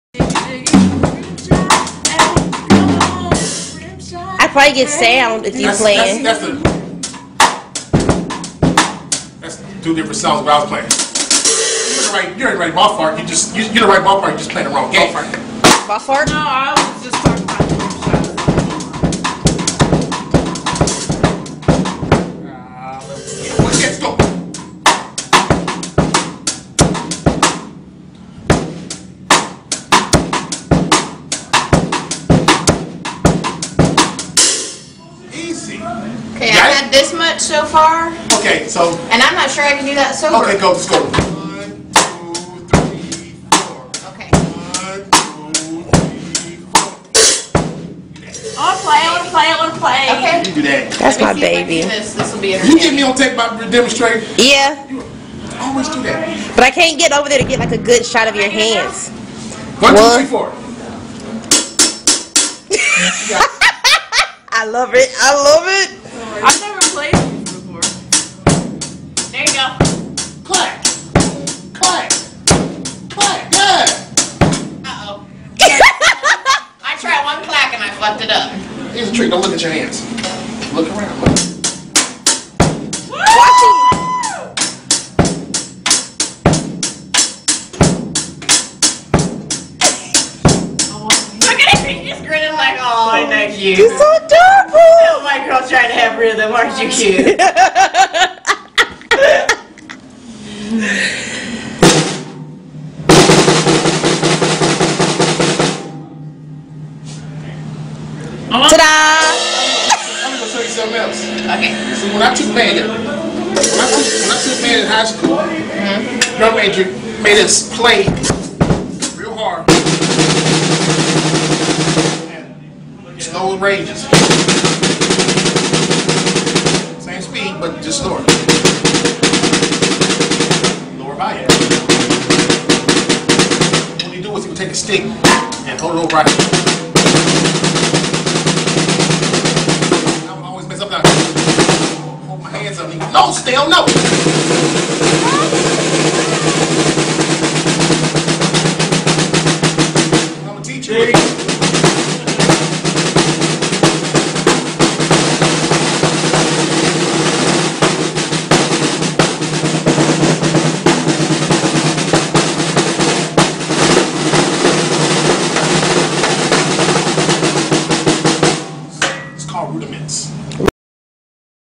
LIFE 3 - THE DRUMMER GUY*his YT name* teaching a tipsy friend how to drum
My best friend just had her birthday on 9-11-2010 and they were playing around with the drums. YES MY MAN CAN REALLY DRUM..he just playing around this night